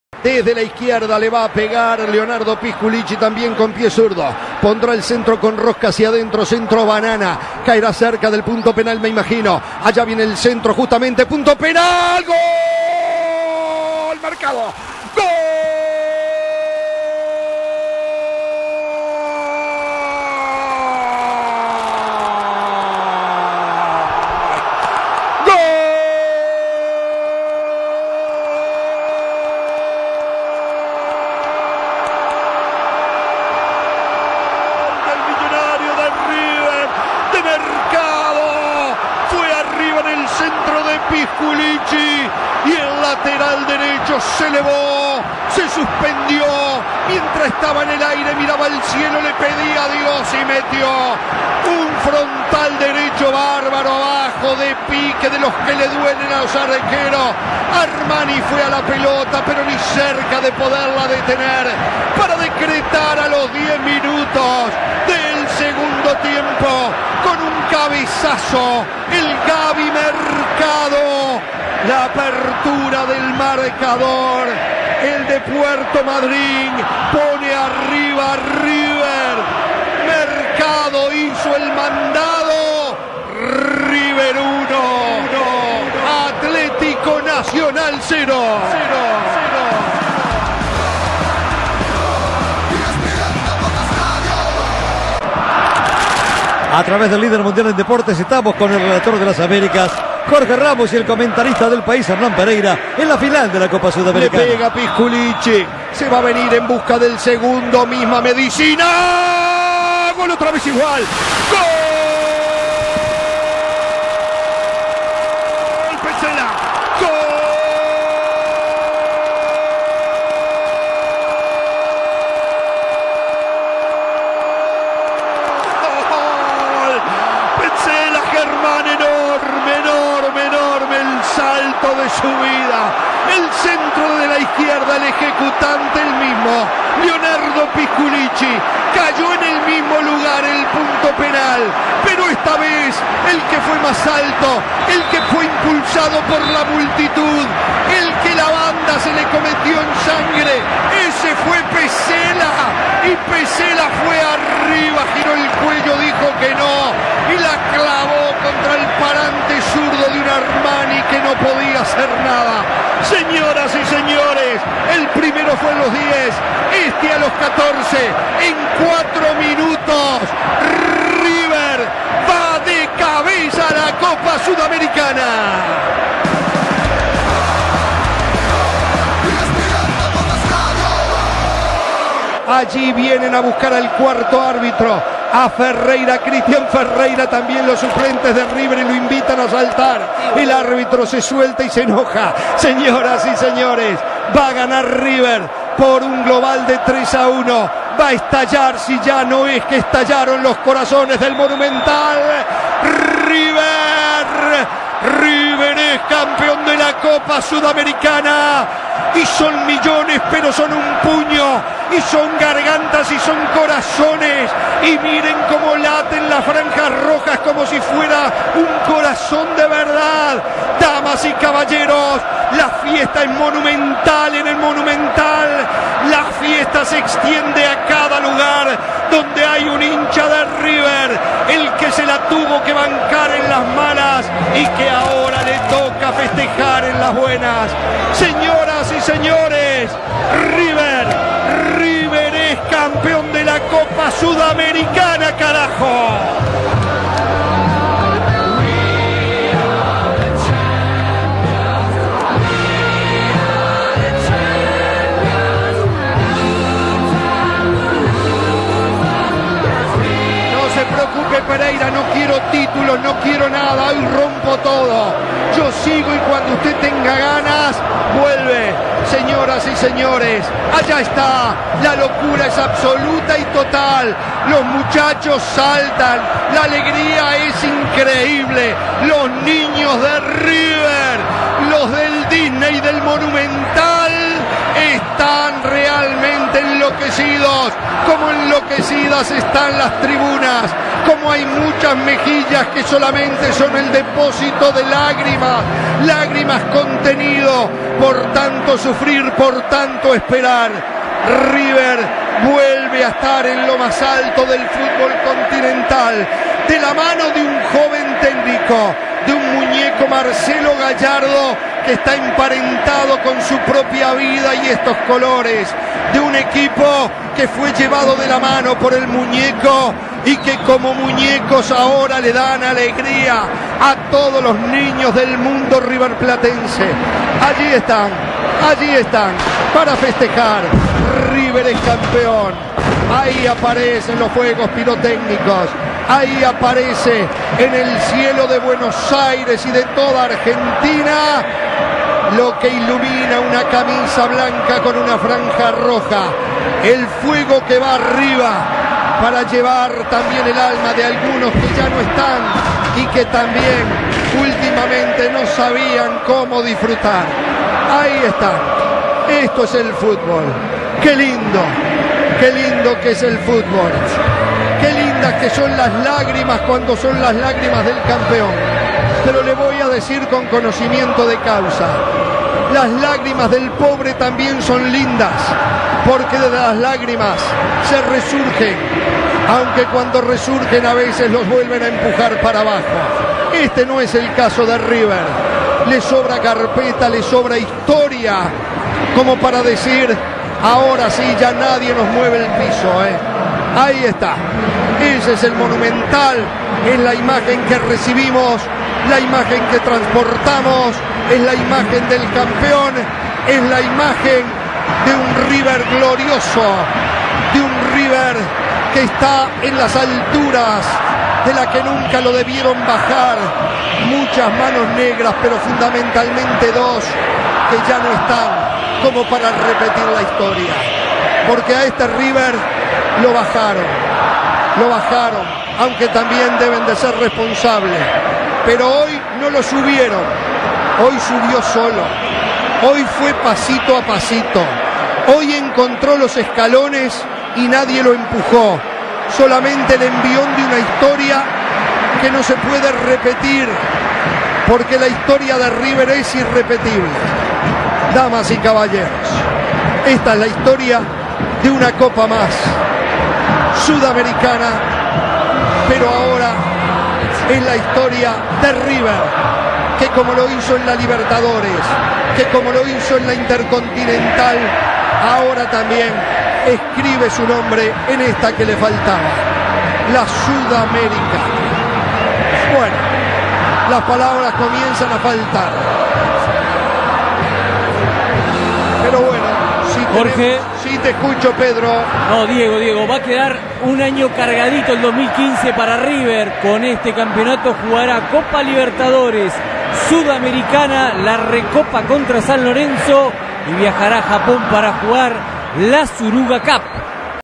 River 2 Atletico Nacional 0 (Espn Deportes Radio) Copa Sudamericana 2014 River Campeon
Los goles del partido con relato de Jorge Ramos Espn Deportes Radio